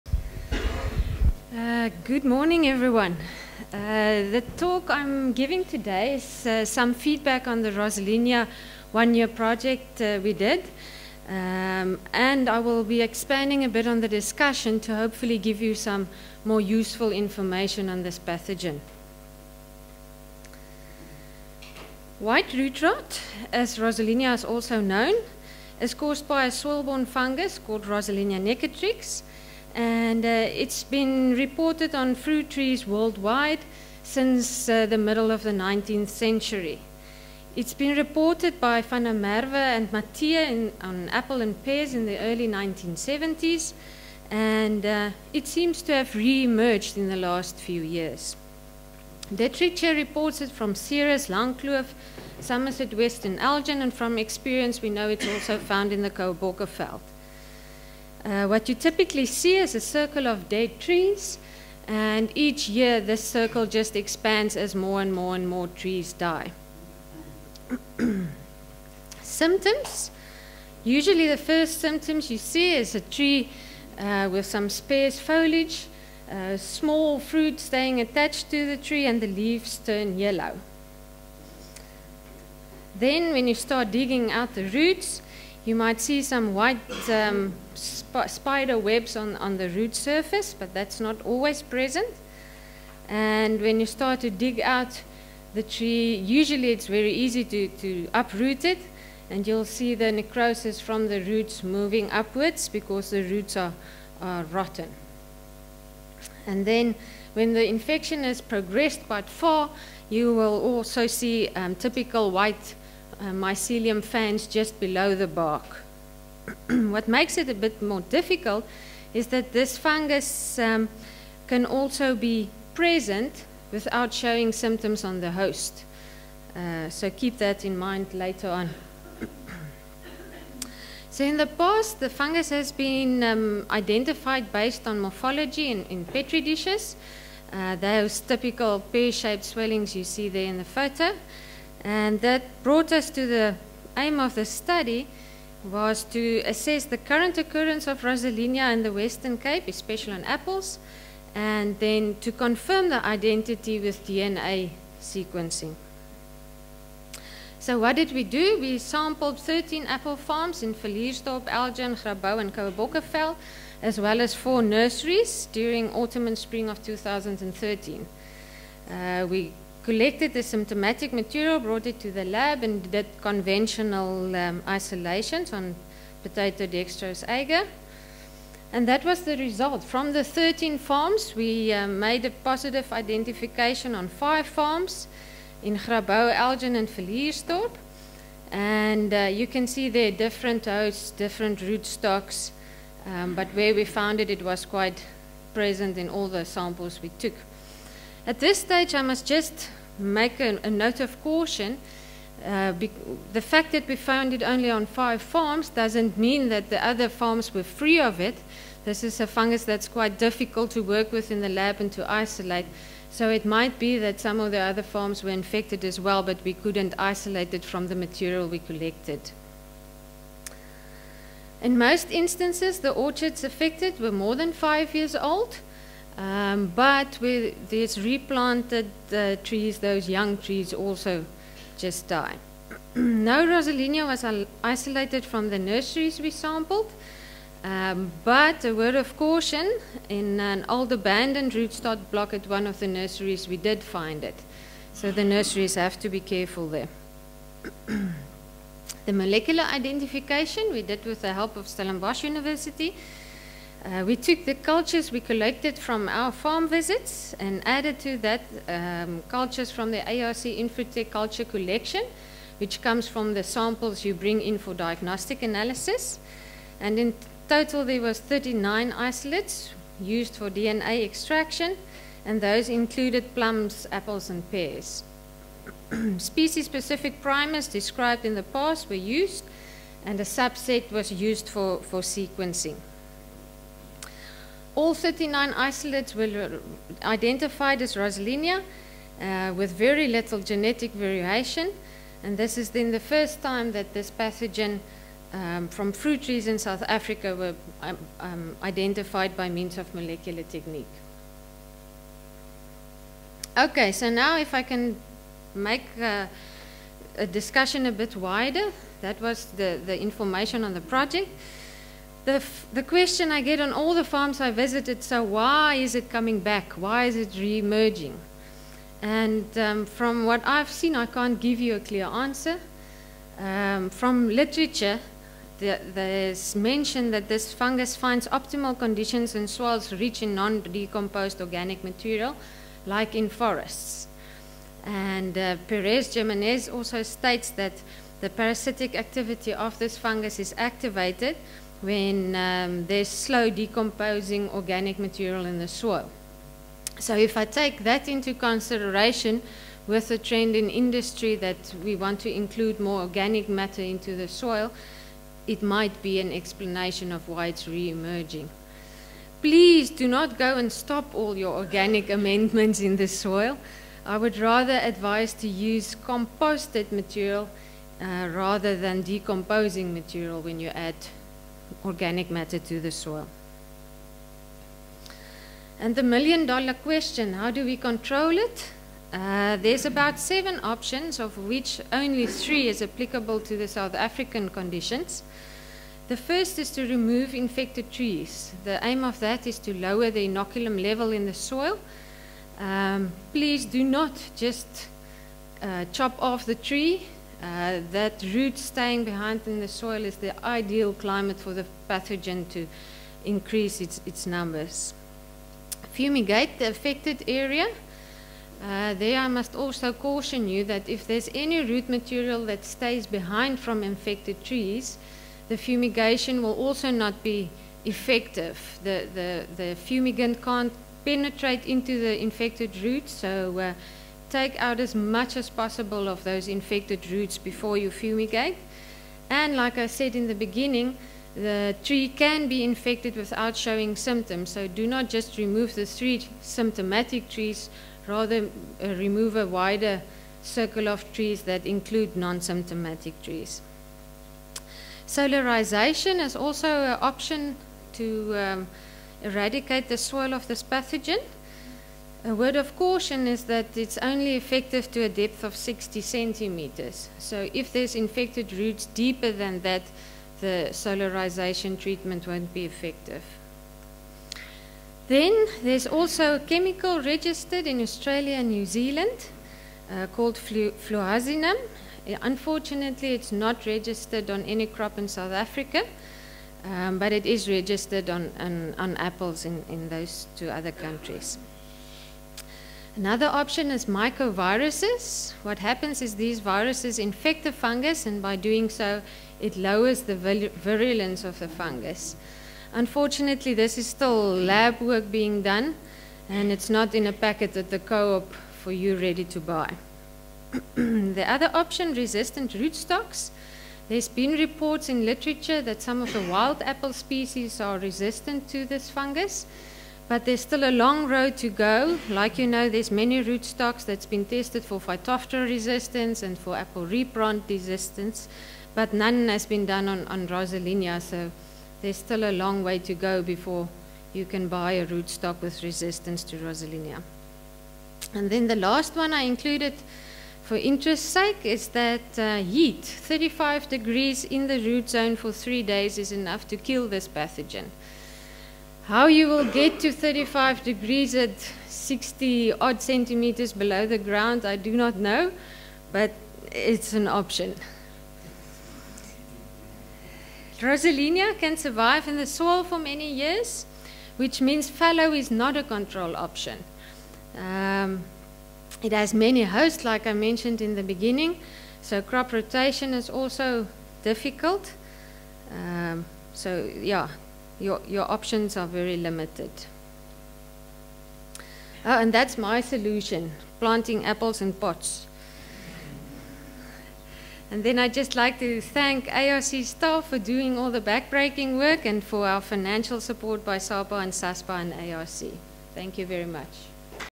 09
Karien Bezuidenhout tackling rosellinia root rot in apples